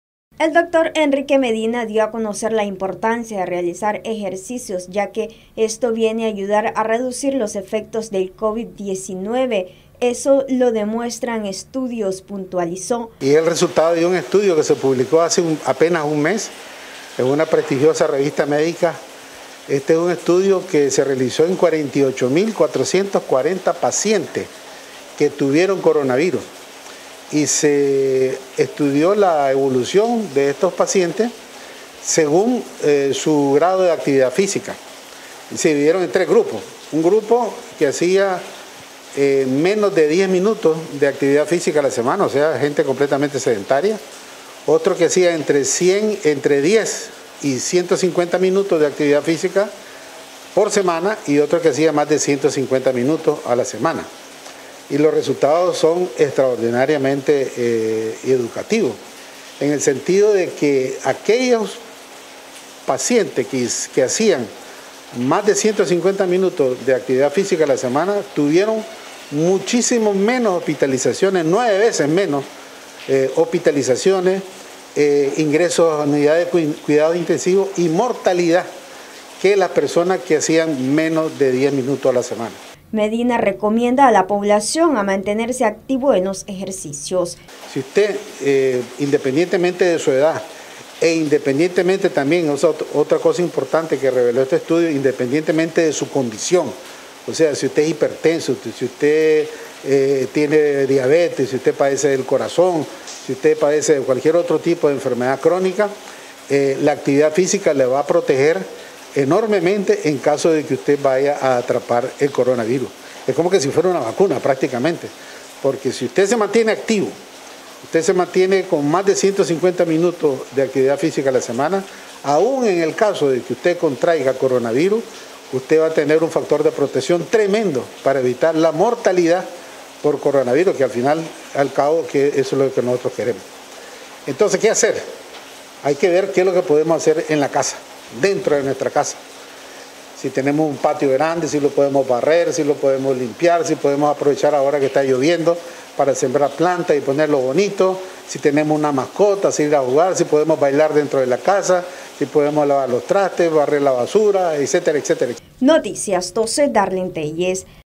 Recomienda a la población que ante el covid-19 es necesario realizar actividades físicas
NosResistimosAlEmbargo Presidente de la Fundación Nicaragüense para la Diabetes, recomienda a la población a realizar actividades físicas, ya que esto viene a reducir riesgo de mortalidad en la persona que se contagie de covid-19 según estudios.